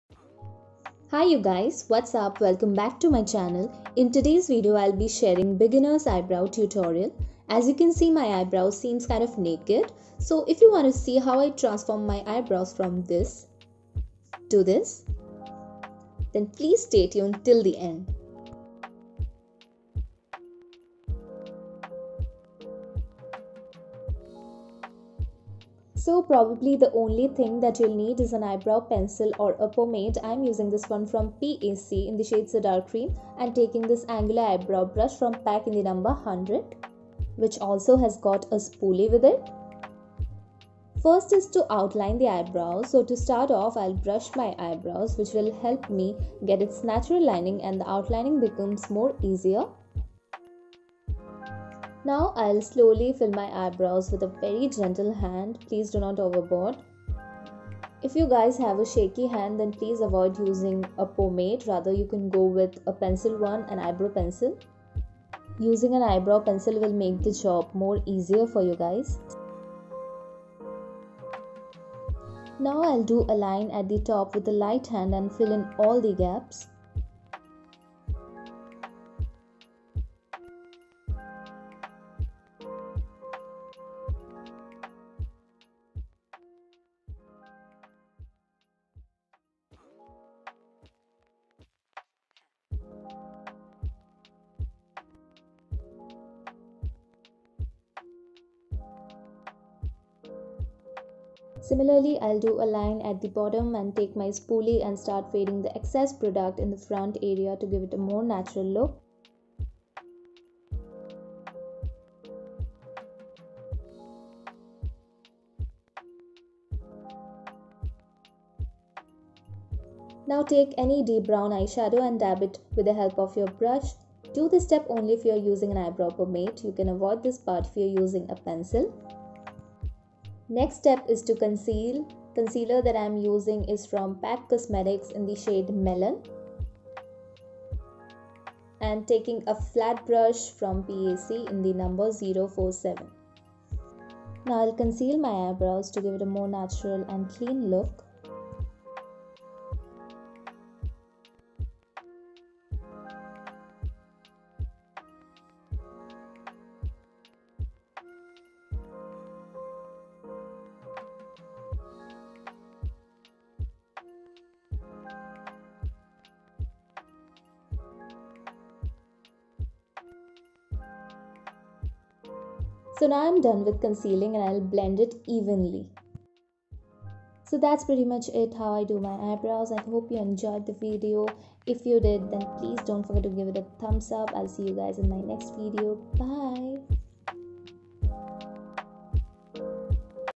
2 Minutes Eyebrow Tutorial | MAKE IT UP
In today's video I have shown how to do eyebrows in few minutes

Musician: Rook1e